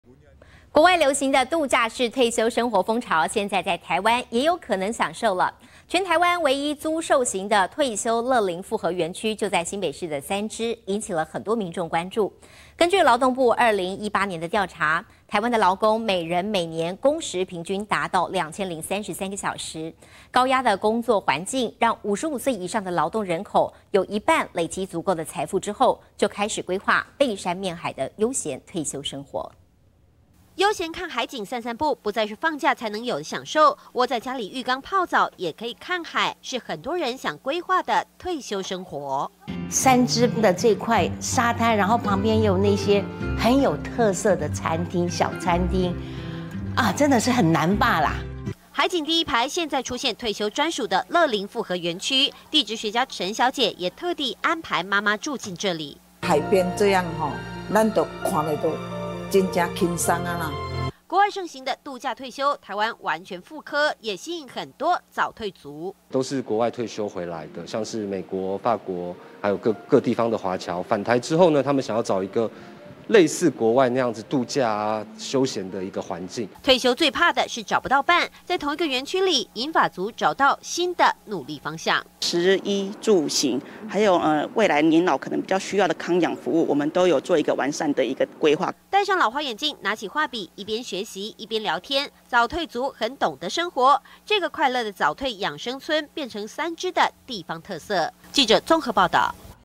悠閒生活不是夢!"退休樂齡複合園區"在三芝
國外流行的度假式退休生活風潮，現在在台灣也有可能享受。全台灣唯一租售型的退休樂齡複合園區， 就在新北市三芝，引起很多民眾關注，根據勞動部2018 的調查，台灣勞工每人每年工時平均達2，033 小時。高壓工作環，讓55歲以上的勞動人口，有一半累積足夠財富之後，就開始規劃背山面海的悠閒退休生活。